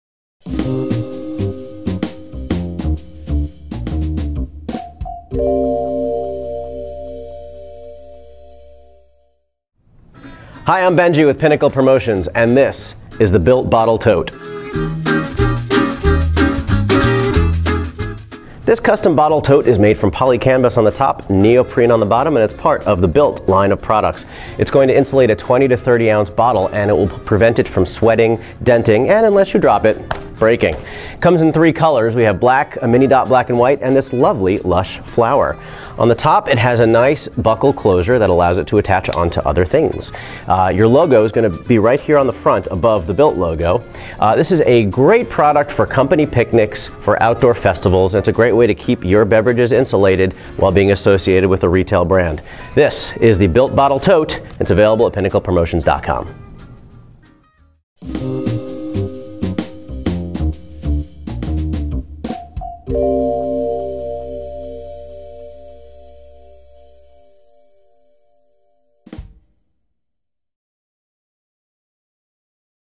BUILT Rolltop Bottle Tote 20-30 Ounce Video
BUILT Rolltop Bottle Tote 20-30 Ounce by Pinnacle Promotions
This custom bottle tote is made from polycanvas and neoprene and is part of the BUILT line.  It will insulate a 20-30 oz bottle and prevent it from sweating, denting and breaking.  It features a unique buckle closures that allows it to attach to other bags.  The tote comes in three color choices - black, mini polka-dots and flowers - and your logo will be imprinted above the BUILT logo.  Great for picnics, tailgates and outdoor festivals.